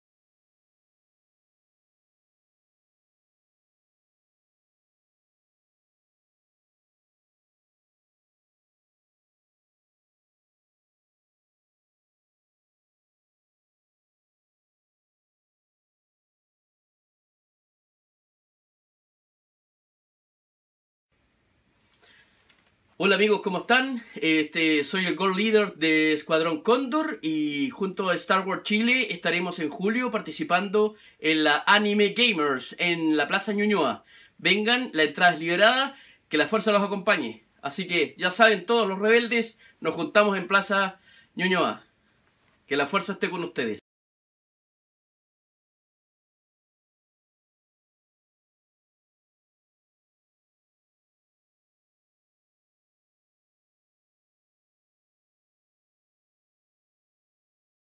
ANIGAMER FEST Y STARWARSCHILE
El Fan Club starwarschile esta invitado por la I.Municipalidad de ÑUÑOA el 15-16-17 de Julio al ANIGAMER FEST en Plaza Ñuñoa Entrada Liberada.